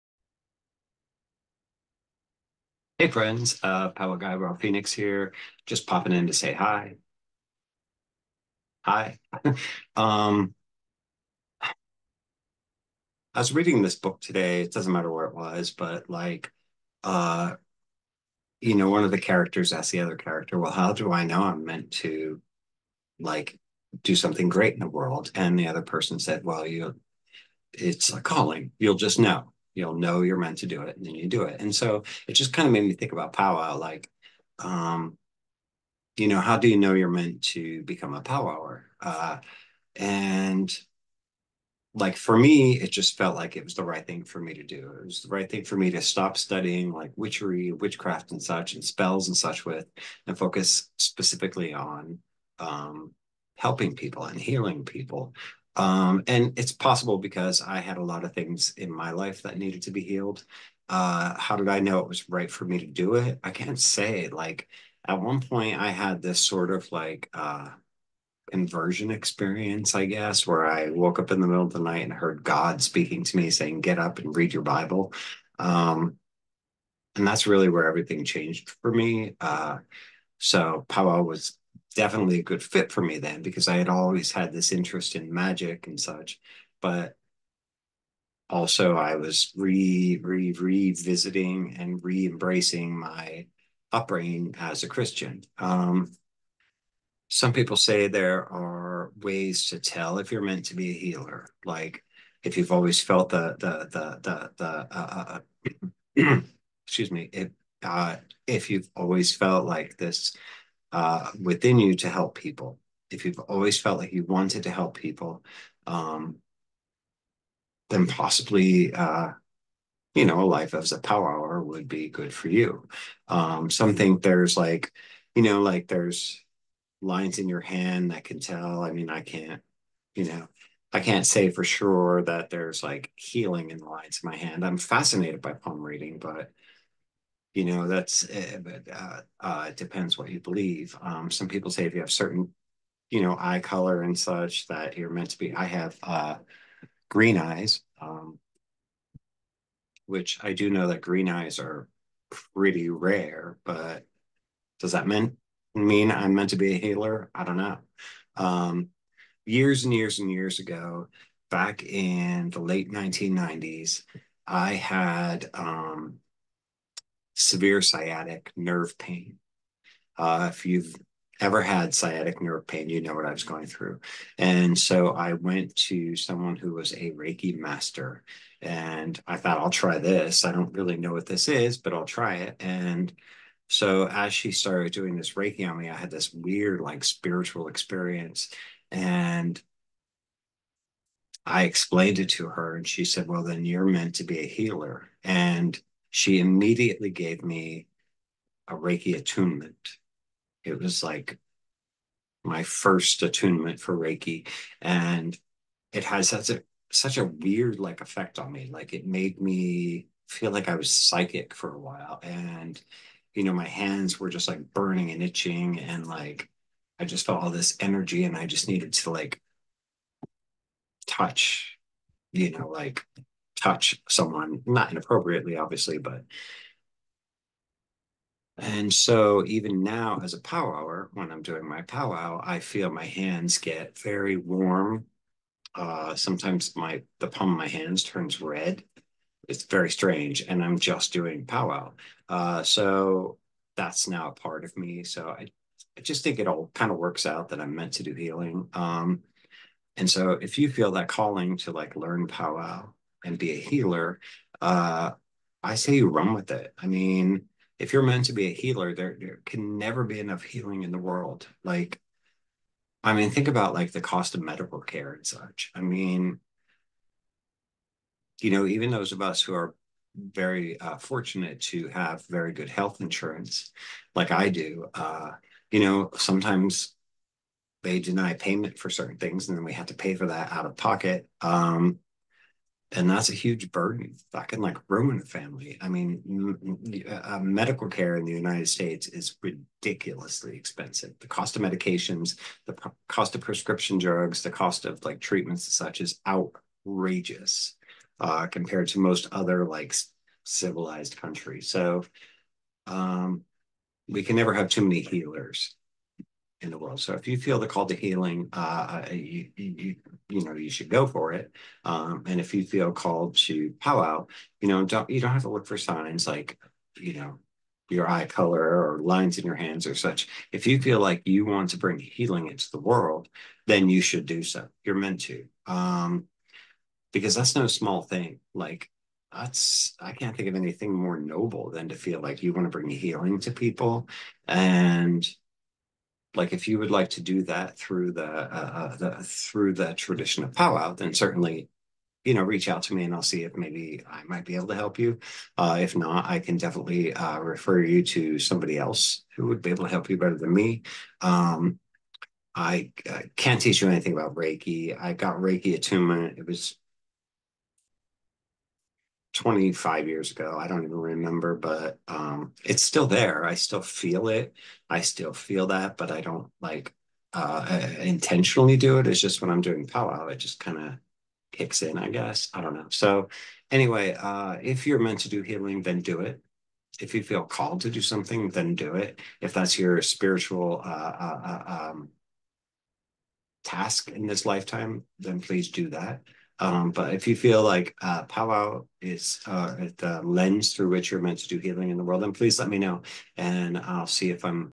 The Calling to be a Healer
I talk about the various ways in which some of us are called to be healers in this world. I'm sorry the video sort of cuts off abruptly at the end, not sure what I did wrong :P